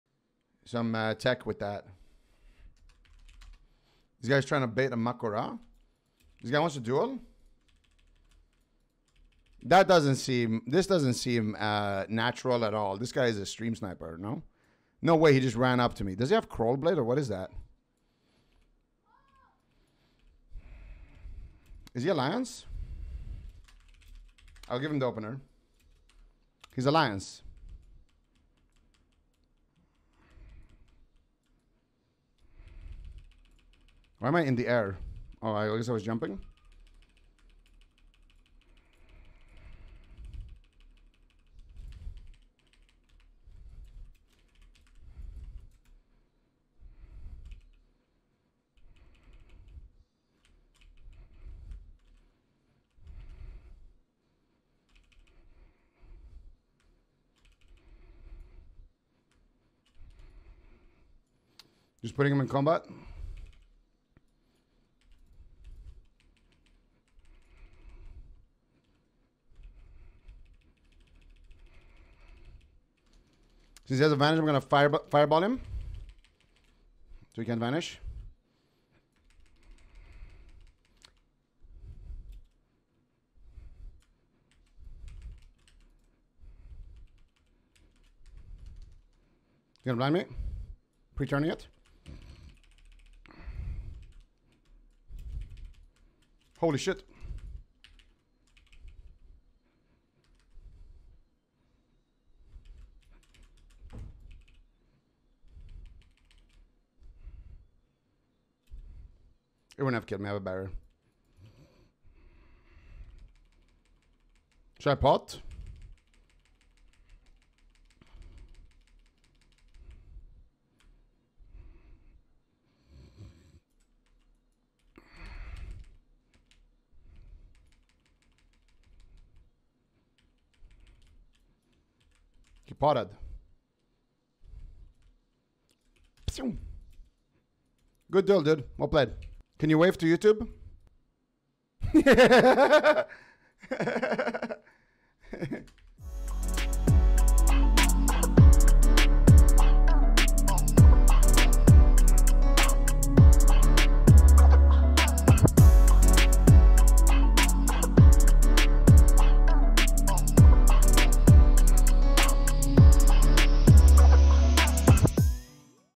This guy challenged me while on the road *mage vs rogue duel*
This guy was like the highway man, just on the road wanting to duel. Did we send him?

Socials:

Instagram.com/Ziqoftw
Twitter.com/Ziqoftw

PEACE!